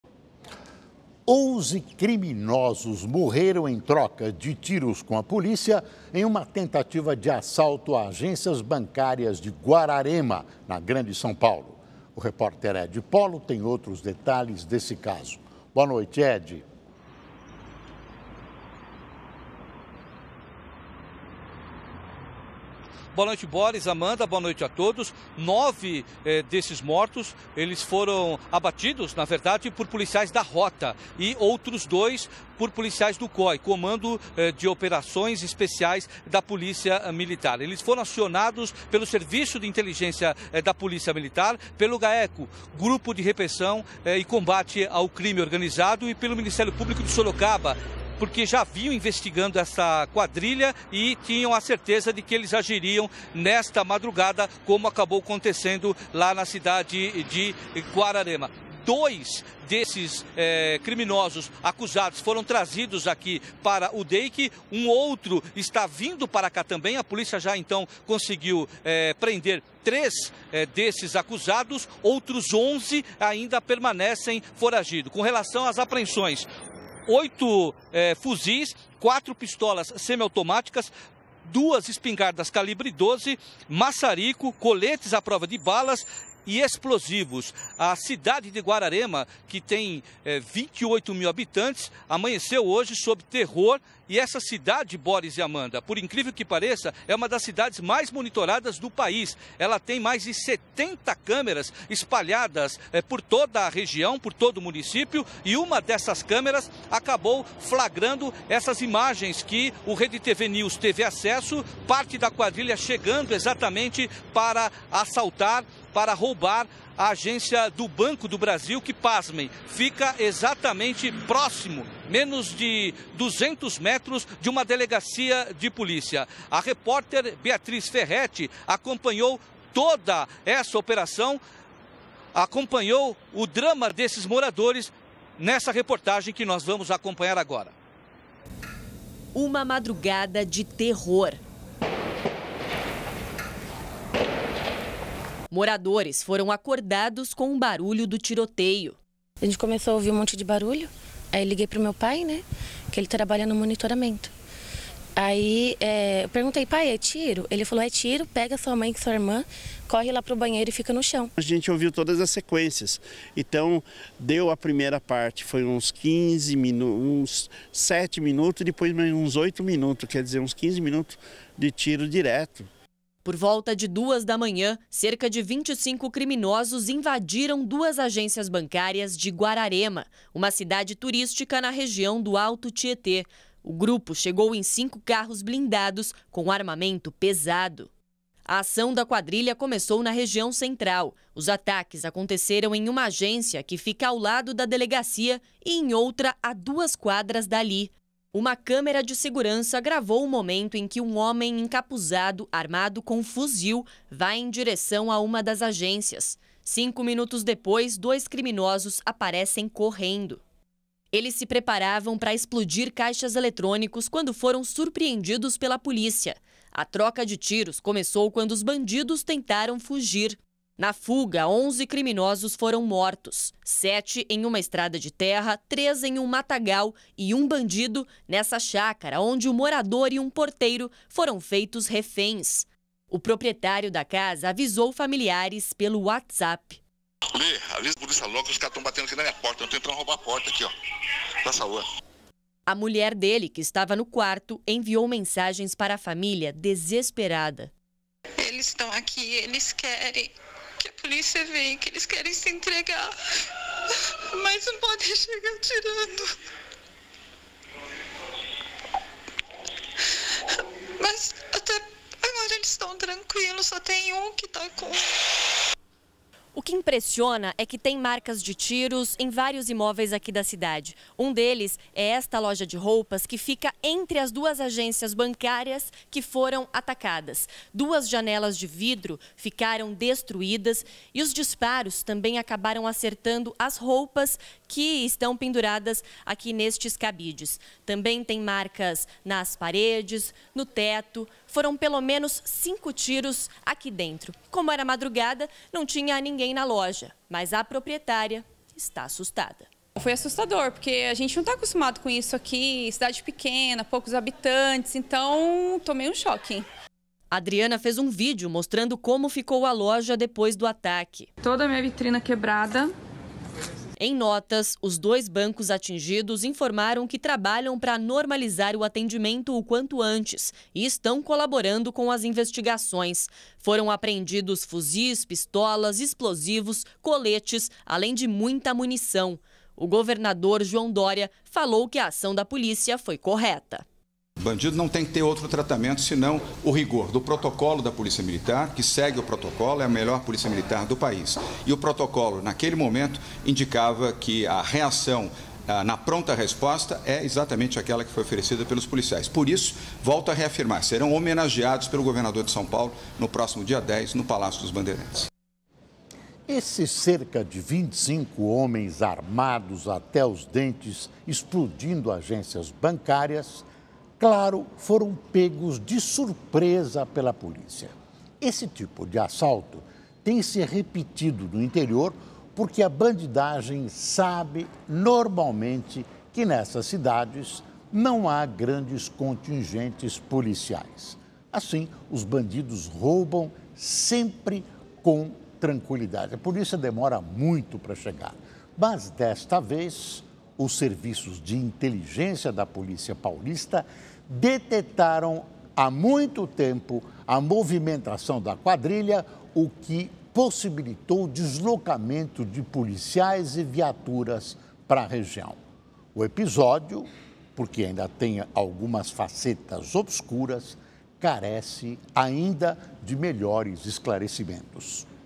11 mortos em tentativa de assalto a banco em Guararema, outros três foram presos
A secretaria de Segurança Pública de São Paulo informou, em coletiva no começo da tarde desta quinta-feira (4), que três criminosos estão presos no DEIC, na Zona Norte da capital, e pelo menos 11 estão foragidos. Com a quadrilha foram apreendidos oito fuzis, quatro pistolas semiautomáticas, duas de calibre 12, um maçarico, coletes a prova de balas e vários explosivos. Os bandidos usaram, no assaltom cinco veículos, dois deles blindados.